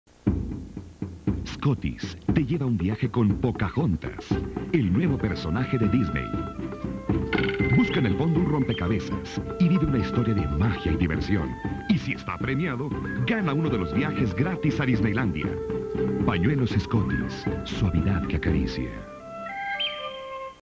Comerciales mexicanos: Scottis 1995
Pañuelos desechables que incluyen rompecabezas de la pelicula Pocahontas